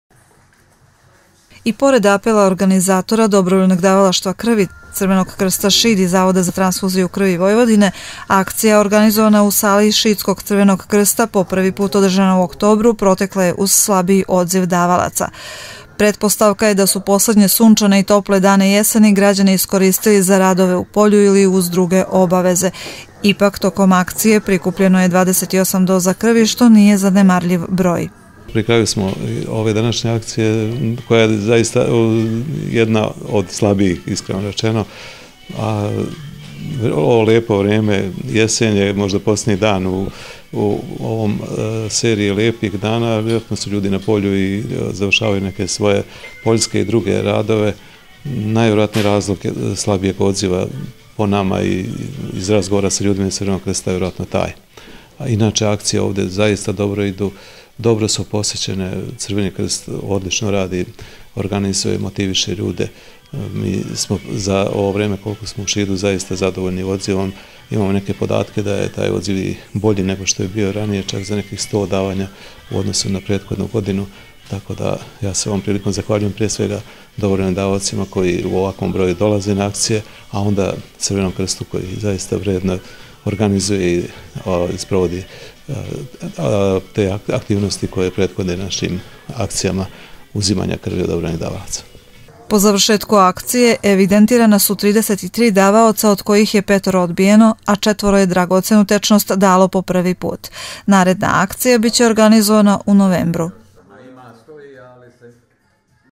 DDK Šid
U Šidu je danas održana redovna akcija dobrovoljnog davalanja krvi.Akcija je protekla po planu ali sa nešto slabijim odzivom davalaca